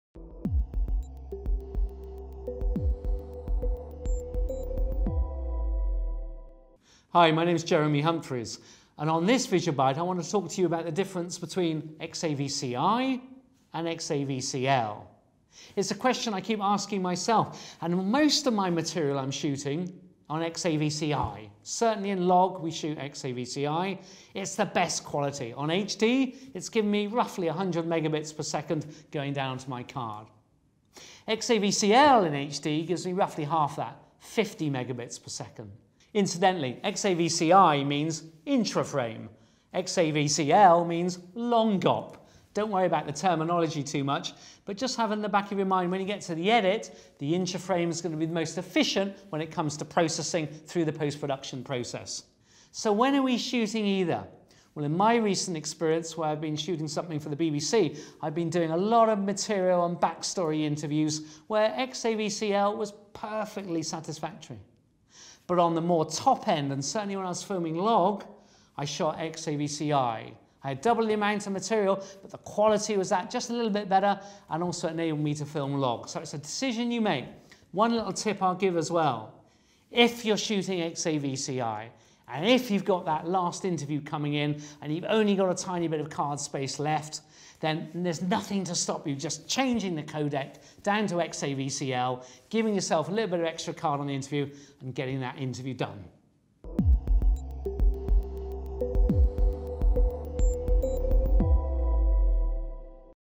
Visual Bytes - 07 XAVC-I vs XAVC-L
What's the difference between XAVC-I & XAVC-L and when you should use which one